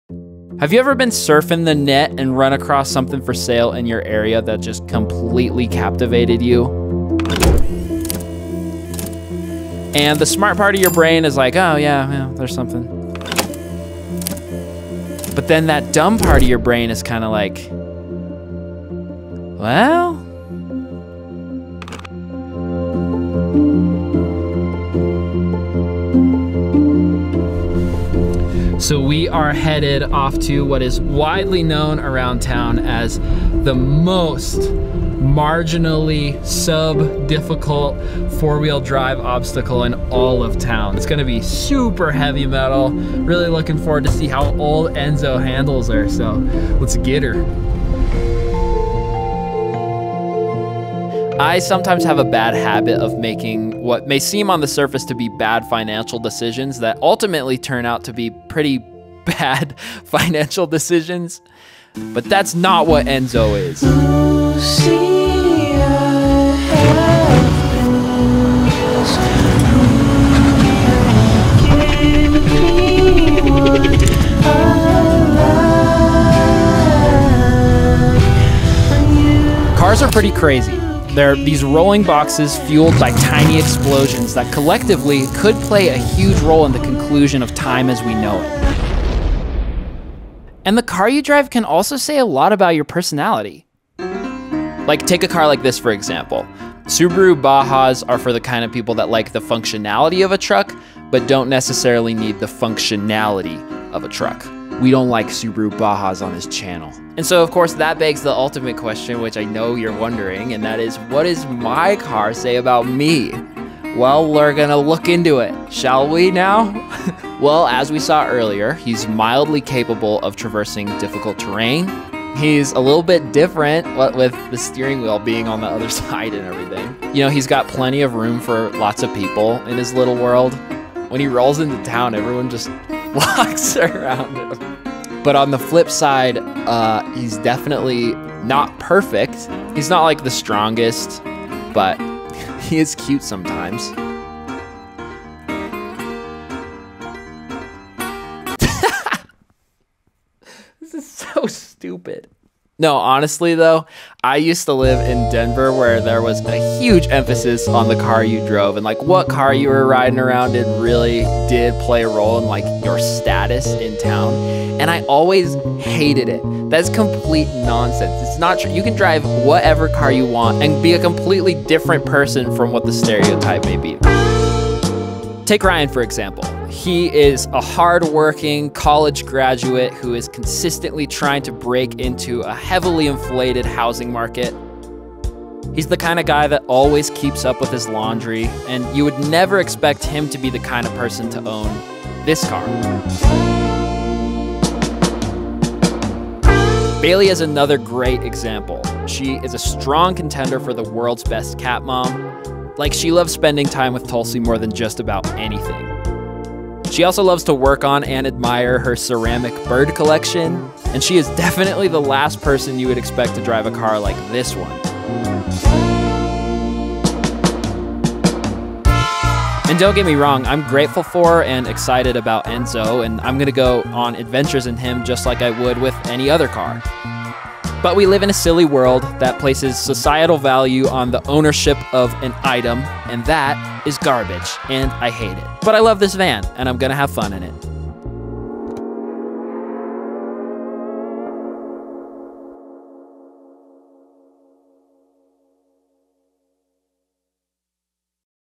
My minivan camper - what it says about me...
My new/old JDM minivan camper says a lot about me, what does your car say about you? Long live the Toyota townace and longlvne doing whatever you want with your short, short life.

Thanks for the help!
Ryan: (@rockymountainodyssey)
Bailey: (@bailsos)

What my minivan camper says about me...